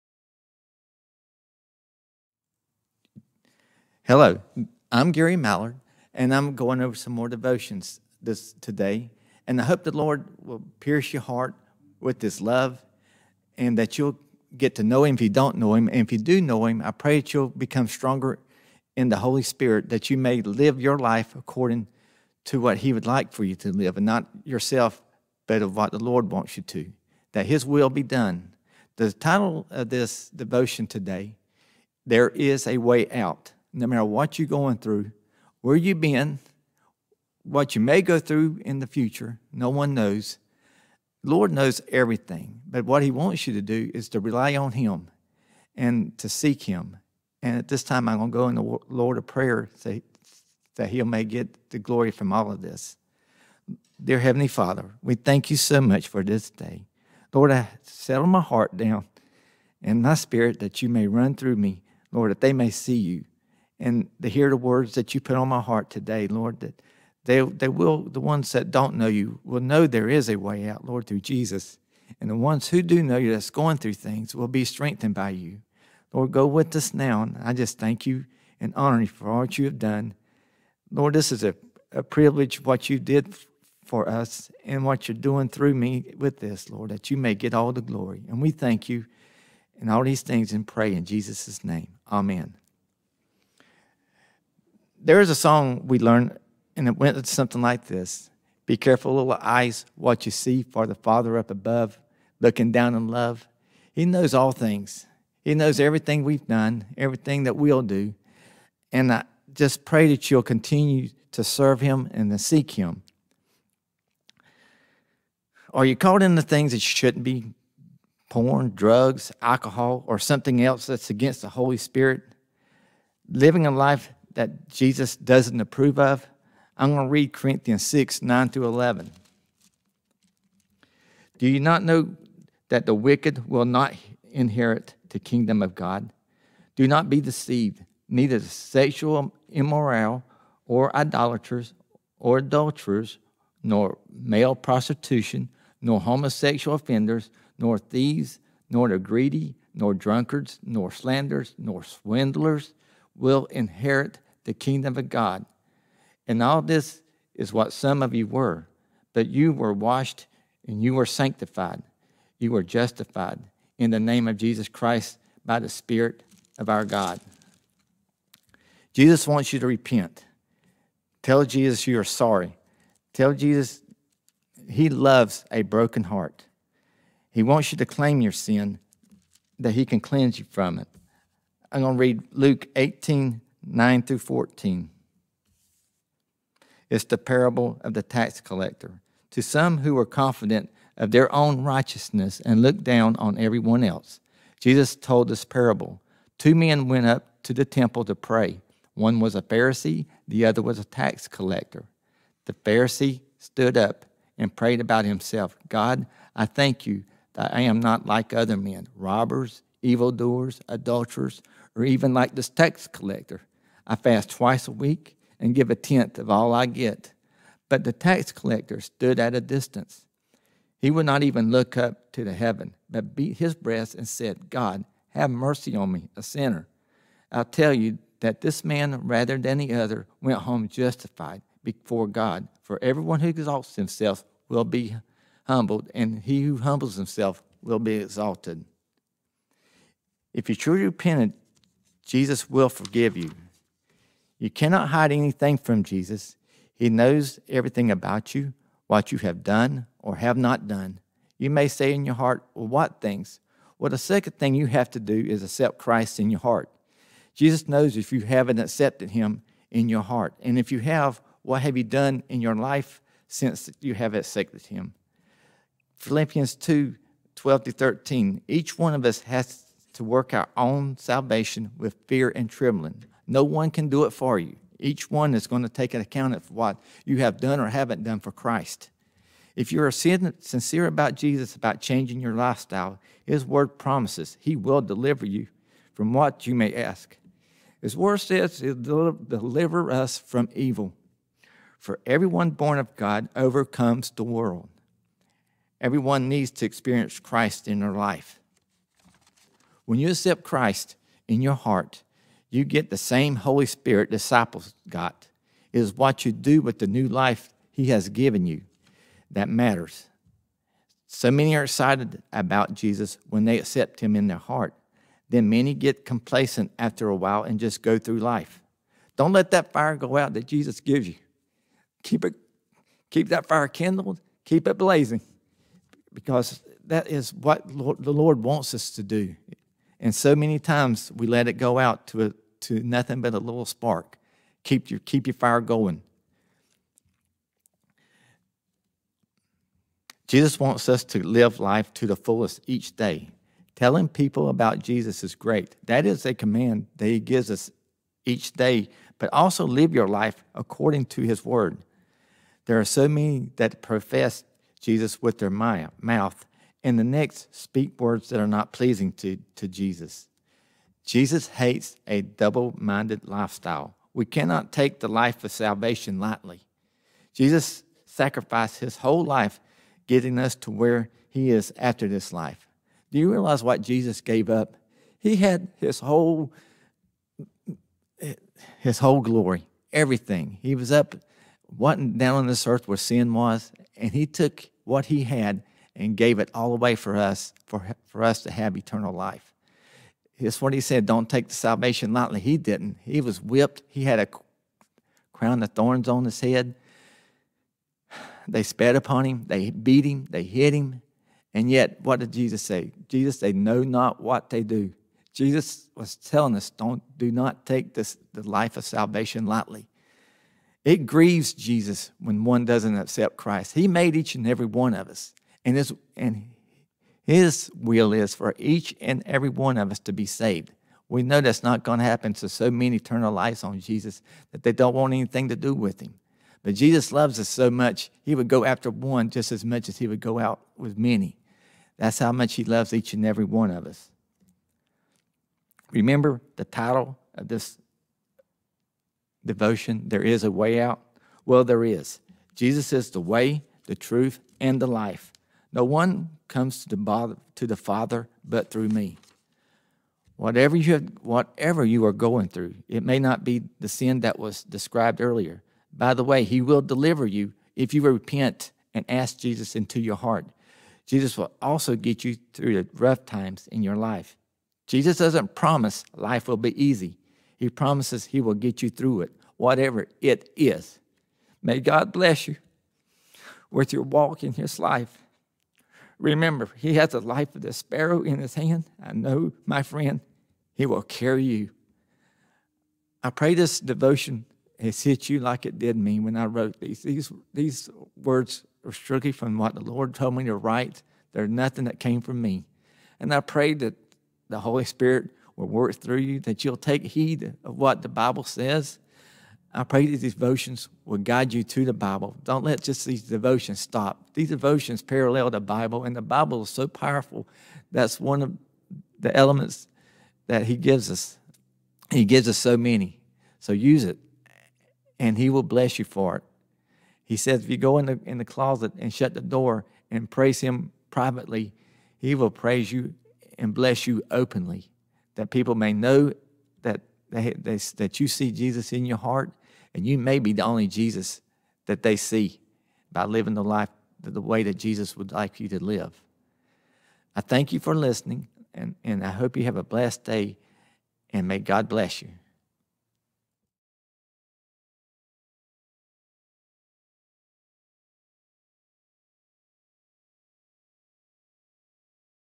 "There Is A Way Out" devotional message with Gary
1 Corinthians 6:9-14; Luke 18:9-14